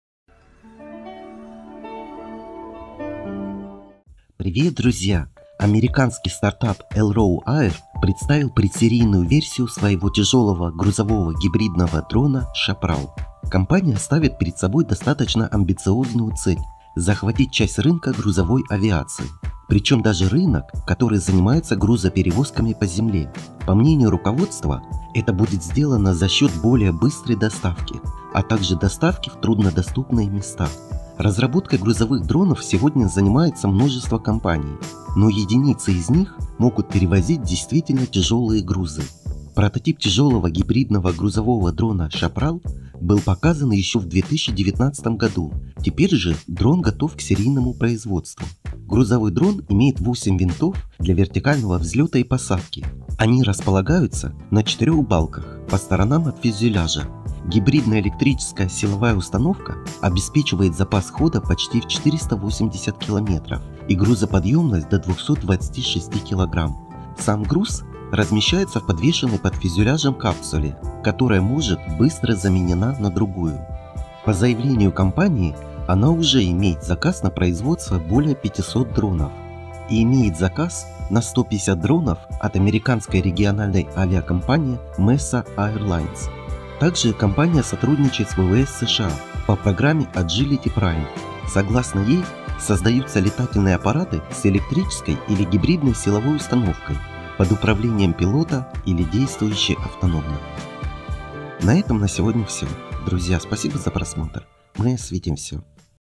Тяжелый грузовой дрон Chaparral заменит грузовую авиацию
Дрон для захвата рынка грузовой авиации показали в США. Американский стартап Elroy Air представил предсерийную версию своего тяжелого грузового гибридного дрона Chaparral. Компания ставит перед собой достаточно амбициозную цель, захватить часть рынка грузовой авиации, причем даже рынок, который занимаются грузоперевозками по земле, по мнению руководства это будет сделано за счет более быстрой доставки, а также доставки в труднодоступные места. Разработкой грузовых дронов сегодня занимается множество компаний, но единицы из них могут перевозить действительно тяжелые грузы. Прототип тяжелого гибридного грузового дрона Chaparral был показан еще в 2019 году, теперь же дрон готов к серийному производству. Грузовой дрон имеет восемь винтов для вертикального взлета и посадки. Гибридно-электрическая силовая установка обеспечивает запас хода почти в 480 километров и грузоподъёмность до 226 кг. Сам груз размещается в подвешенной под фюзеляжем капсуле, которая может быстро заменена другую. По заявлению компании, она уже имеет заказ на производство более 500 тяжелых дронов Chaparral и имеет заказ на 150 дронов от  американской региональной авикомпании Mesa Airlines. Также компания сотрудничает  с ВВС США по программе Agility Prime, согласно ей, создаются летательные аппараты с электрической или гибридной силовой установкой, под управлением пилота, или действующие автономно.

Источник Elroy Air